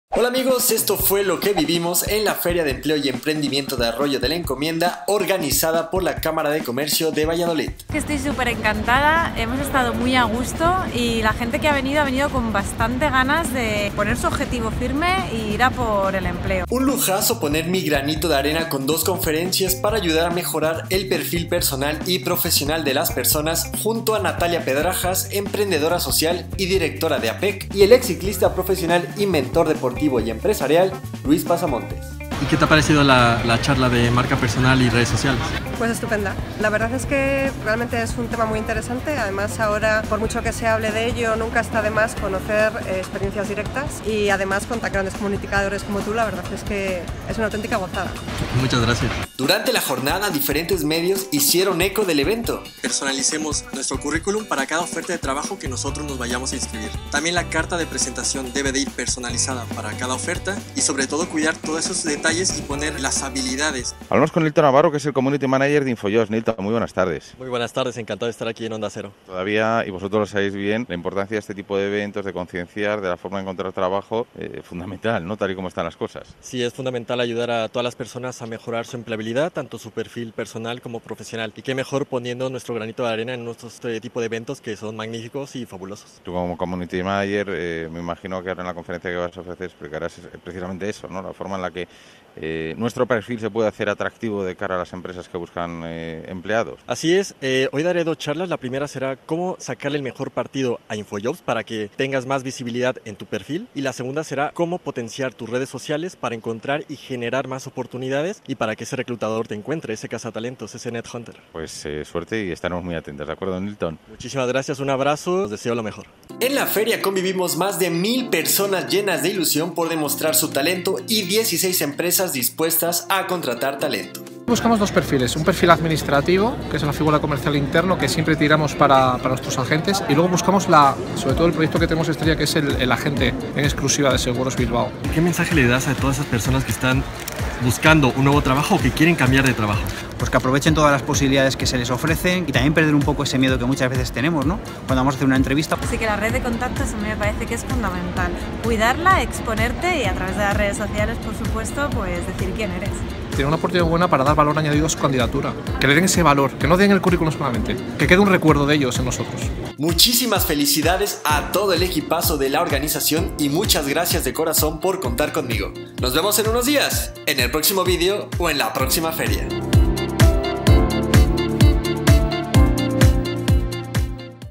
+1.000 personas en la Feria de Empleo Valladolid | Redes Sociales | InfoJobs | Nilton Navarro
Me ha encantado estar con más de 1.000 personas en la Feria de Empleo y Emprendimiento organizada por la Cámara de Comercio de Valladolid y el Ayuntamiento de Arroyo de la Encomienda.

¡Muchas gracias por contar conmigo! ;-)

El 20 de Septiembre estuve en Valladolid ilusionando y ayudando a mejorar el perfil personal y profesional de los asistentes. En las charlas explique cómo sacar mayor partido a InfoJobs y a las RedesSociales para potenciar la MarcaPersonal y conseguir el trabajo deseado.

▂ ▃ ▅ ▆ █ SÍGUEME █ ▆ ▅ ▃ ▂